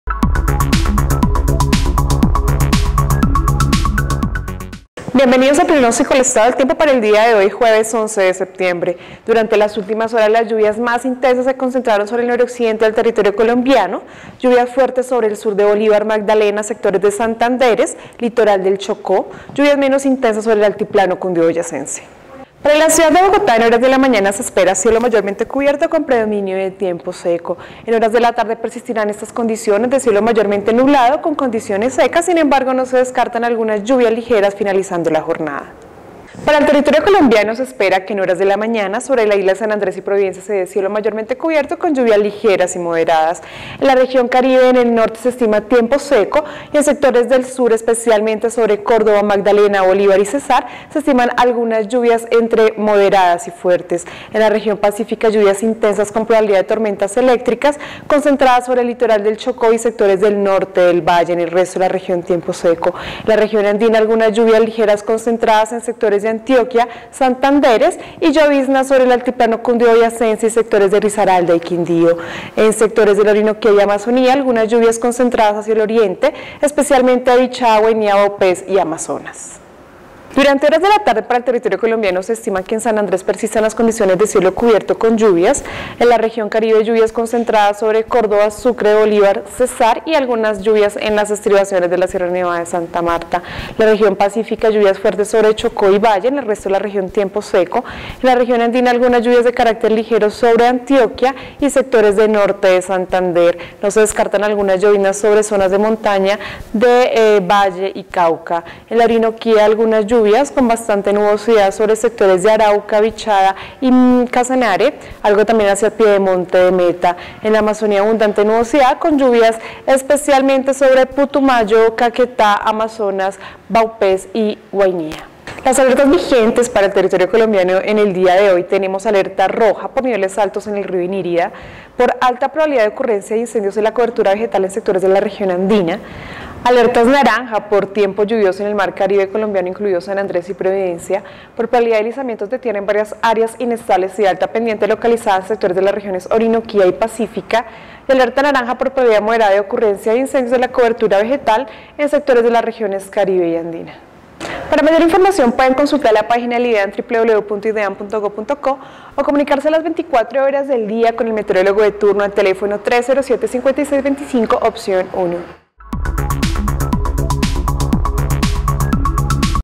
Pronóstico del Tiempo - Jueves 11 de Septiembre de 2014
Pronóstico del tiempo virtual para el Jueves 11 de Septiembre de 2014, anunciando condiciones Hidrometeorológicas  en Colombia.